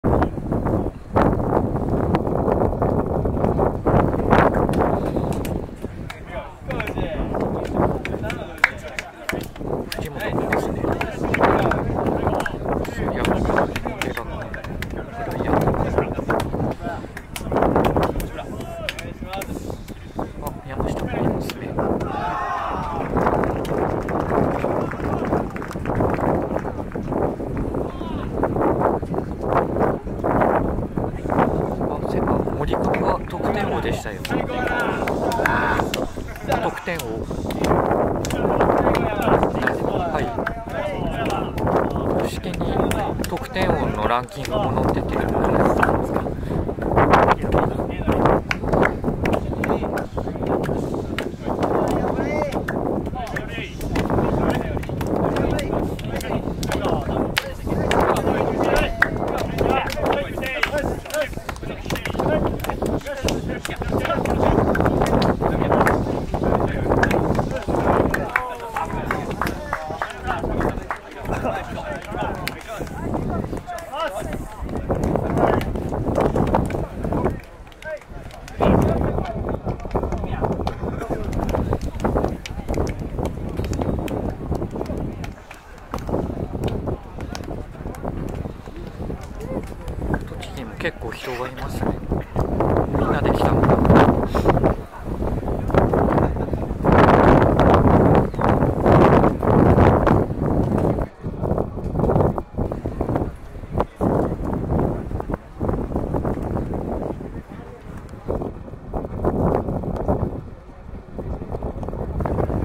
2018/10/27 第26回Jリーグユース選手権 3回戦 栃木SC戦(時之栖スポーツセンター(裾野グラウンド)) 0-1